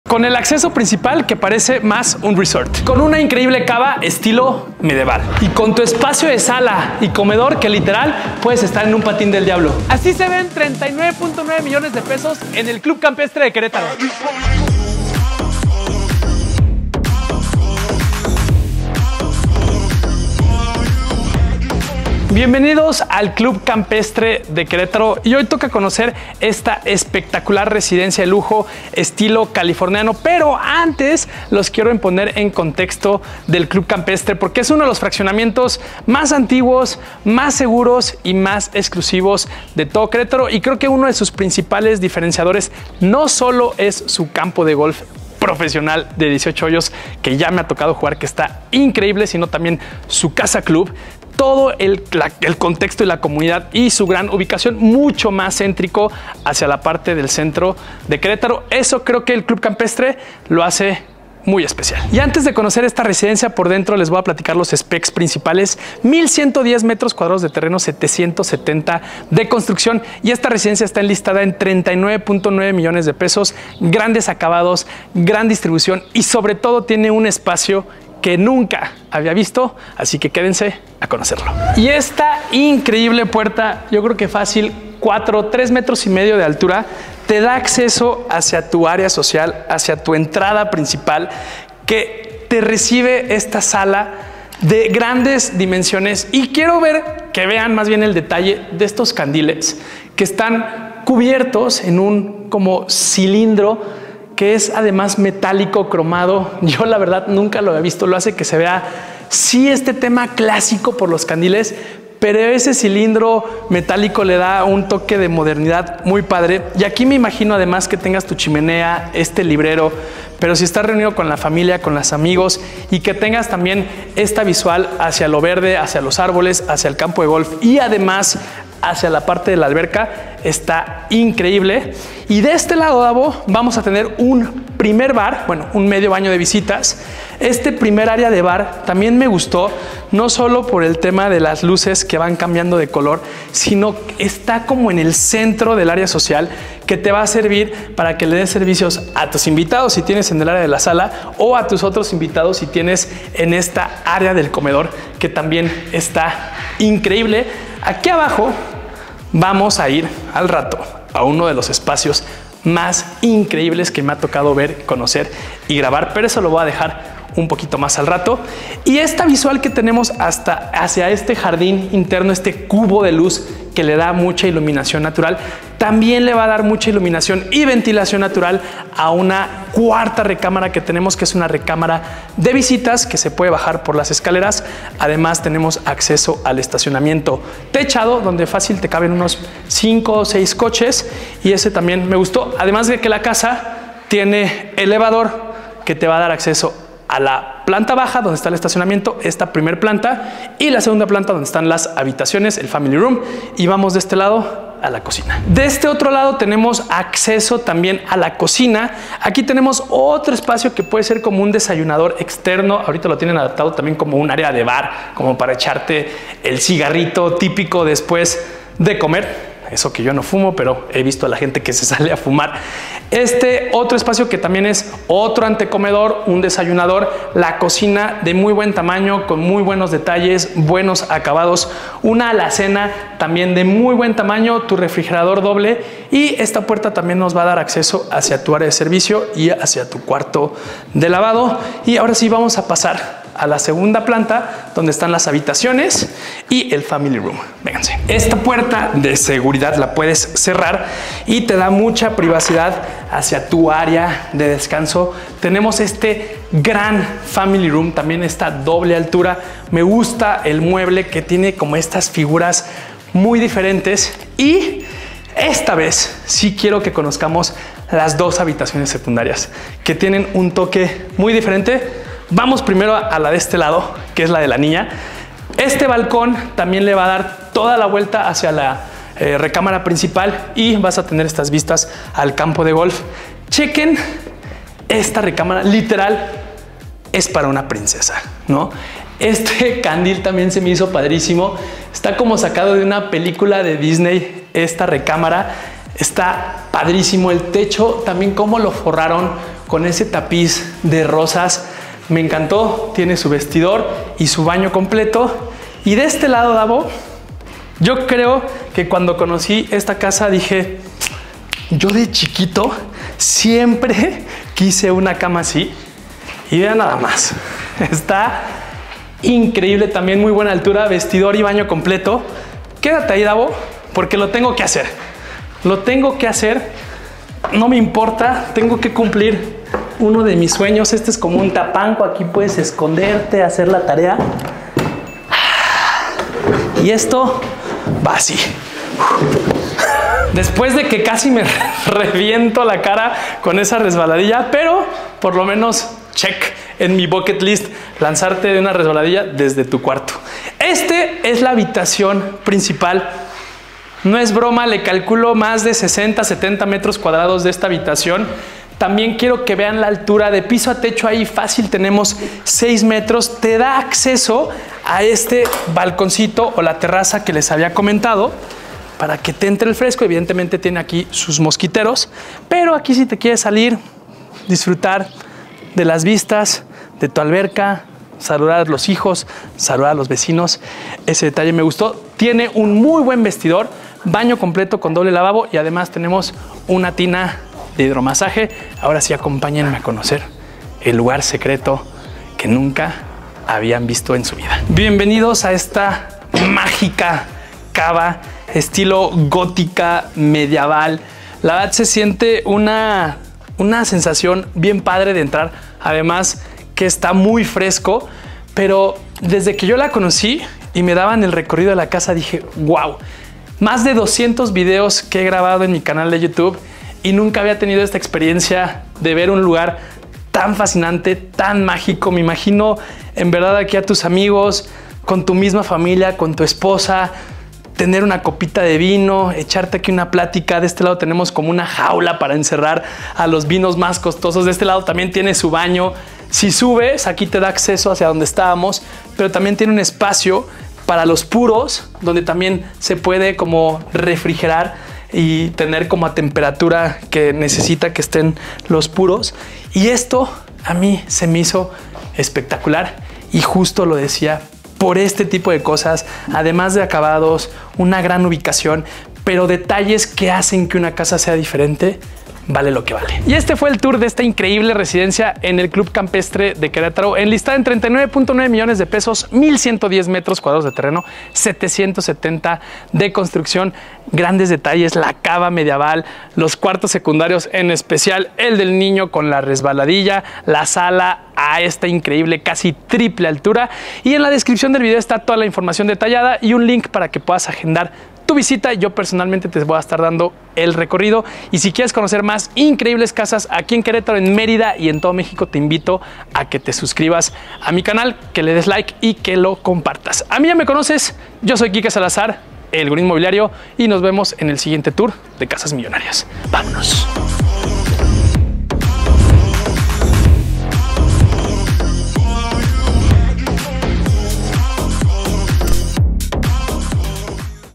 Casa De LUJO en Fraccionamiento EXCLUSIVO | $39,900,000 | Club Campestre Querétaro
Casa Campestre  - Club Campestre Querétaro

Enlistada en $39,900,000

Bienvenido a Casa en Club Campestre, Querétaro. Una residencia de lujo con un estilo californiano que tiene muchas sorpresas.